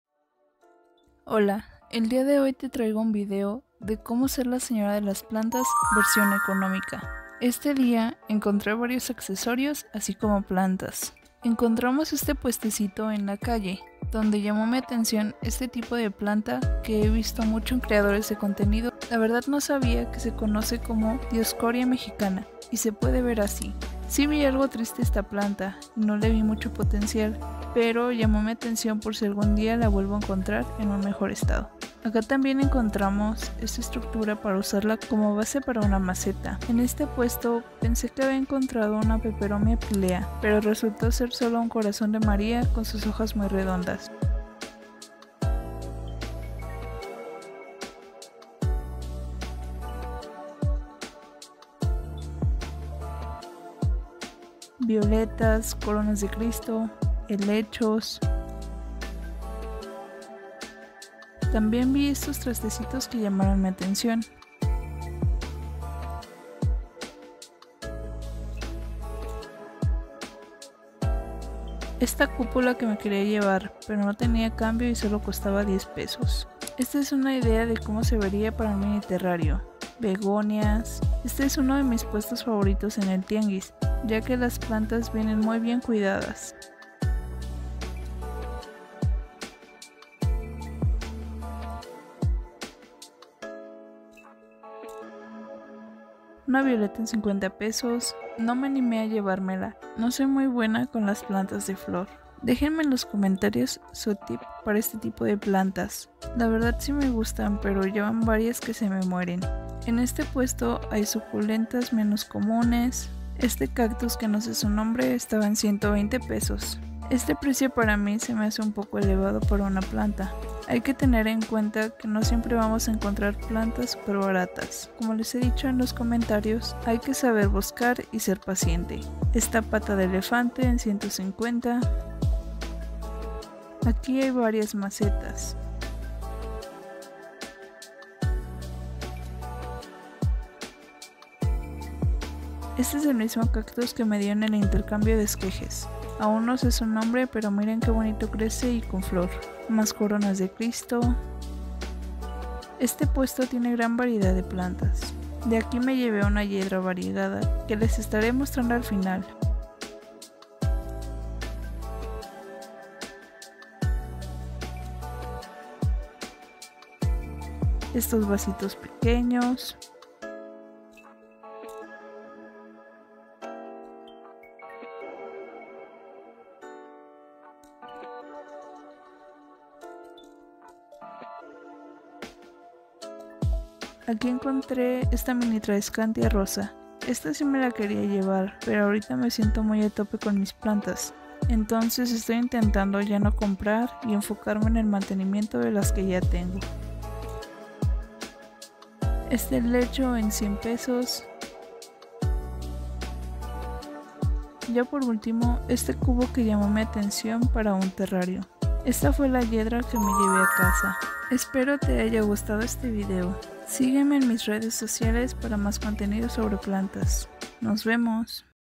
Como ser la señora de las plantas versión económica parte 3 🫰✨🧺🪴
Acompáñame al tianguis de mi ciudad en búsqueda de plantas y accesorios para plantas, espero te guste :).